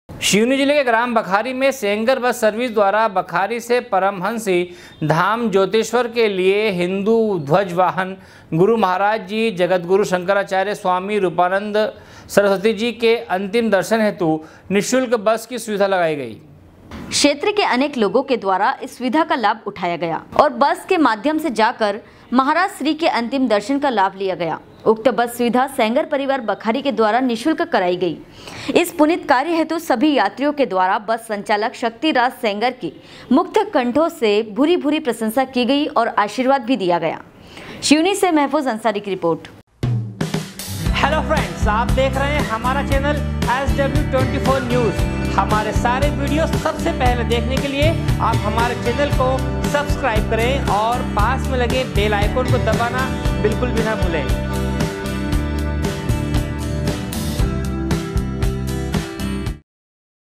सिवनी जिले के ग्राम बखारी से सेंगर बस सर्विस द्वारा बखारी से परमहंसी धाम जोतेश्वर के लिये निःशुल्क
सिवनी जिले के ग्राम बखारी से सेंगर बस सर्विस द्वारा बखारी से परमहंसी धाम जोतेश्वर के लिये हिन्दू ध्वज वाहक गुरू महाराज श्री जगद्गुरु शंकराचार्य स्वामी स्वरूपानंद सरस्वती जी के अंतिम दर्शन हेतु निःशुल्क बस की सुविधा लगाई गई सिवनी से महफूज अंसारी की रिपोर्ट